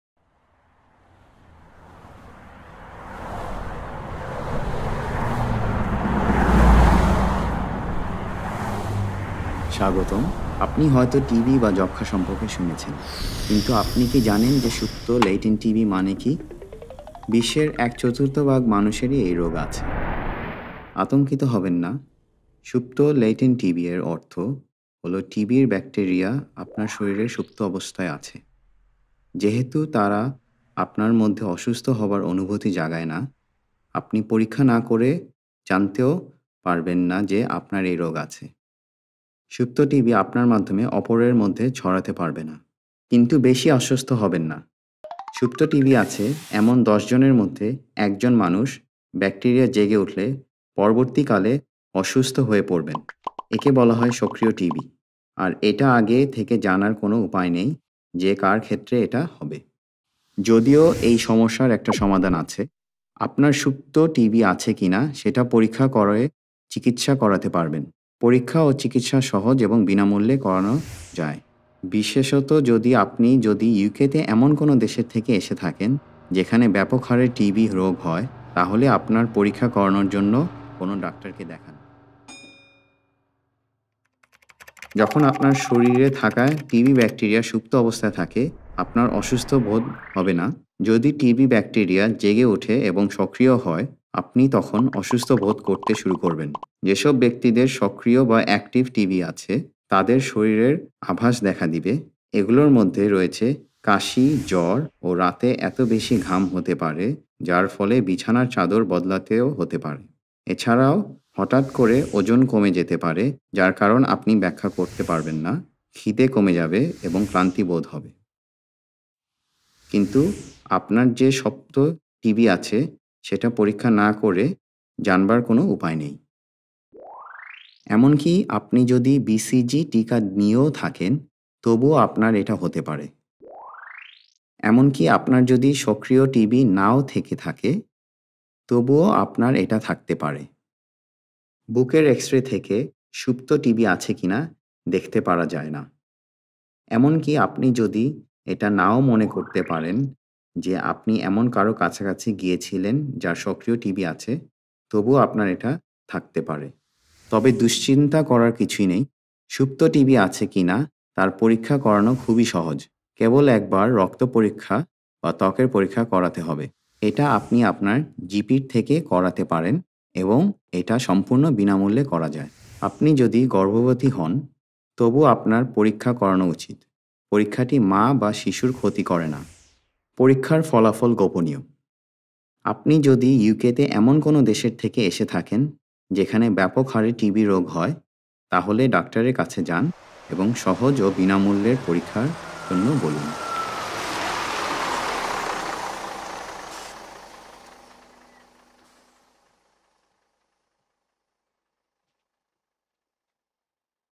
Latent TB: animated into action - Bengali
Latent tuberculosis: Get your free test.

Latent tuberculosis (TB): animated into action is a new health promotion tool that informs people about latent TB. A short animated film, its goal is to engage new migrants from countries with high rates of TB with local NHS testing and treatment programmes.

Members of TB-affected communities in East London were invited to participate in the design of the animation. This provided the development team with a crucial understanding of their knowledge and attitudes towards TB, aiding the development of messages around the benefits of testing and treatment for a hidden—and too often misunderstood—condition.

The community engagement work highlighted that TB affected communities often have very little accurate knowledge about TB, and that the illness is often heavily stigmatised. Few have any understanding that TB has a latent form. The animation responds to these challenges with an entertaining, informative message, engaging a broad audience while remaining sensitive to specific cultures and communities.

The project was funded by NHS England and developed by Public Health England, Queen Mary University of London, In Tune For Life and TB Alert. It launched on World TB Day in 2018.